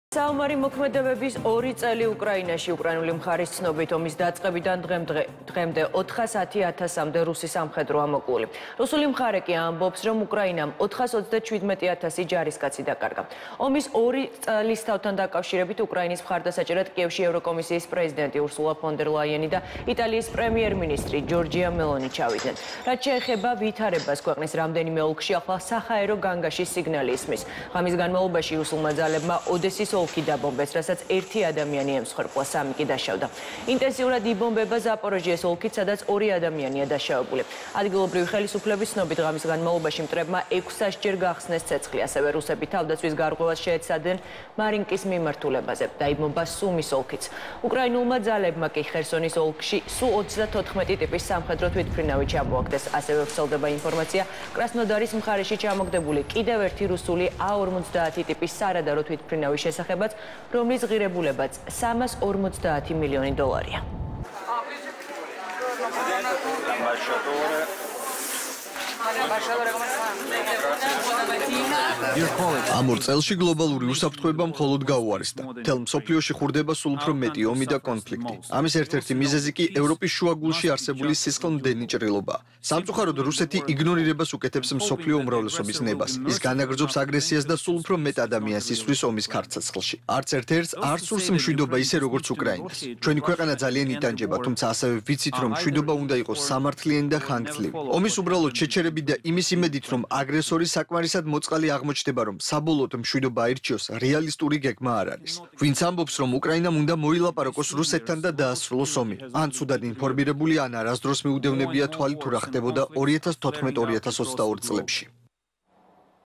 ორი წელი უკრაინაში რუსეთის საომარი მოქმედებების დაწყებიდან
საომარი მოქმედებების ორი წელი უკრაინაში - უკრაინული მხარის ცნობით, ომის დაწყებიდან დღემდე, 410 ათასამდე რუსი სამხედროა მოკლული. რუსული მხარე კი ამბობს, რომ უკრაინამ 437 ათასი ჯარისკაცი დაკარგა. ომის ორი წლისთავთან დაკავშირებით, უკრაინის მხარდასაჭერად, კიევში ევროკომისიის პრეზიდენტი ურსულა ფონ დერ ლაიენი და იტალიის პრემიერ-მინისტრი ჯორჯია მელონი ჩავიდნენ.

რაც შეეხება ვითარებას, ქვეყნის რამდენიმე ოლქში ახლა საჰაერო განგაშის სიგნალი ისმის. ღამის განმავლობაში, რუსულმა ძალებმა ოდესის ოლქი დაბომბეს, რასაც ერთი ადამიანი ემსხვერპლა, სამი კი დაშავდა. ინტენსიურად იბომბება ზაპოროჟიეს ოლქიც, სადაც ორი ადამიანია დაშავებული. ადგილობრივი ხელისუფლების ცნობით, ღამის განმავლობაში მტრებმა 600-ჯერ გახსნეს ცეცხლი. ასევე, რუსები თავდაცვის გარღვევას შეეცადნენ მარინსკის მიმართულებაზე. დაიბომბა სუმის ოლქიც.

უკრაინულმა ძალებმა კი, ხერსონის ოლქში „სუ-34“ ტიპის სამხედრო თვითმფრინავი ჩამოაგდეს. ასევე, ვრცელდება ინფორმაცია კრასნოდარის მხარეში ჩამოგდებული კიდევ ერთი რუსული A-50 ტიპის სარადარო თვითმფრინავის შესახებაც, რომლის ღირებულებაც 350 მილიონი დოლარია.